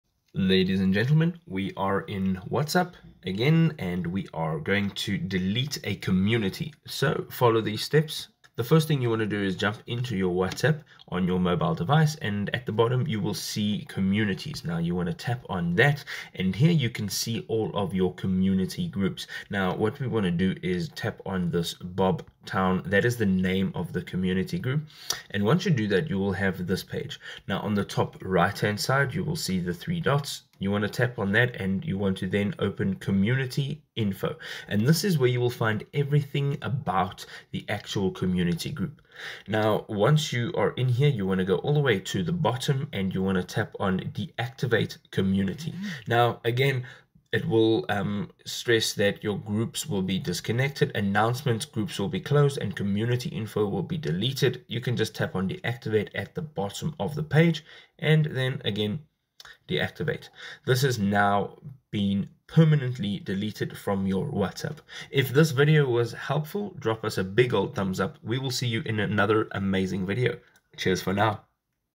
How to Delete Community on WhatsApp | WhatsApp Tutorial 2025
In this video 'How to Delete Community on WhatsApp | WhatsApp Tutorial', we will delve into the process of deleting a community on WhatsApp. Communities in WhatsApp allow users to engage in group chats with shared interests, but there may be instances where you need to remove a community. We will provide you with step-by-step instructions on how to delete a community, ensuring you can manage your group chats effectively and streamline your WhatsApp experience. By mastering this feature, you can declutter your chat list, optimize your group interactions, and maintain control over your WhatsApp communities. Join us as we demonstrate how to delete a community on WhatsApp, empowering you to organize your group chats and enhance your messaging experience on the platform.

Timestamps

Intro – 00:00 – 00:08
Deleting Community on WhatsApp – 00:09 – 01:11